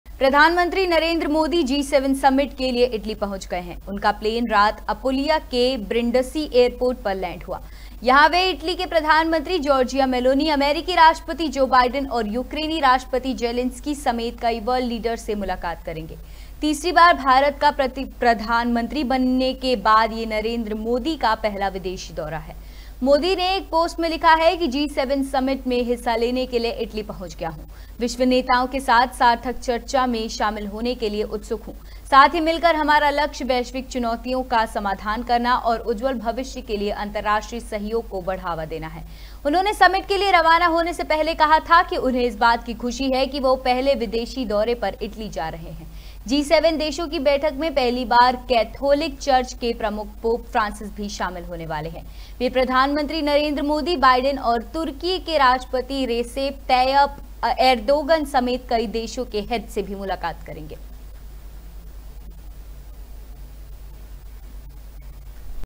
PM Modi In Italy: इटली में मोदी का कुछ इस तरह स्वागत हुआ | Biden | G7 Summit 2024 | Meloni | US
G7 समिट के लिए इटली पहुंचे PM मोदी मेलोनी, पोप फ्रांसिस से मिलेंगे